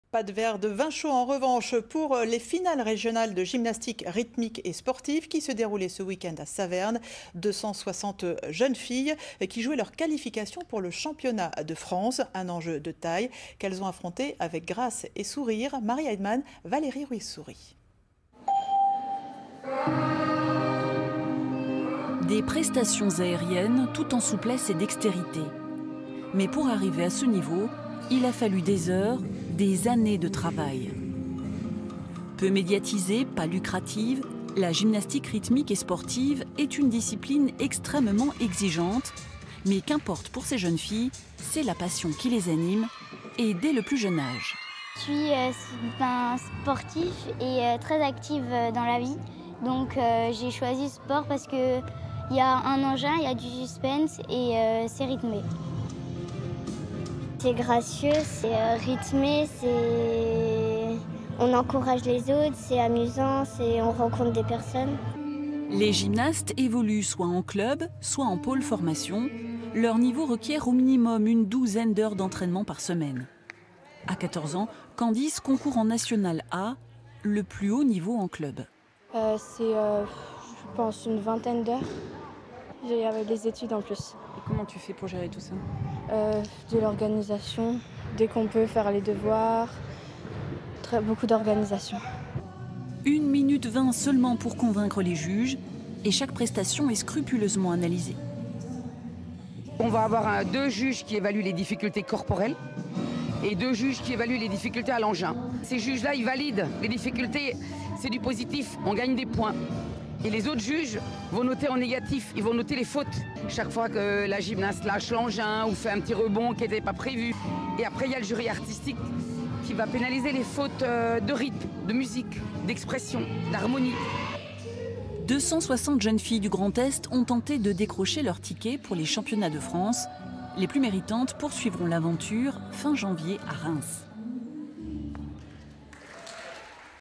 Saverne : 260 filles aux finales régionales de GRS
La gymnastique rythmique sportive compte 38.000 licenciées dans le Grand Est. A Saverne, 260 filles de 10 à 18 ans jouaient leur qualification pour le championnat de France le week-end du 15 décembre. L'enjeu était de taille, mais c'est toujours avec le sourire qu'elles présentaient leur prestation.